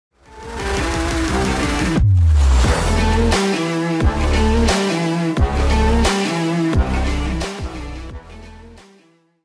BIG FISH SWIMBAIT'N💪🏻
Soft Swimbait big toothy fish in the backwaters of the Mississippi River!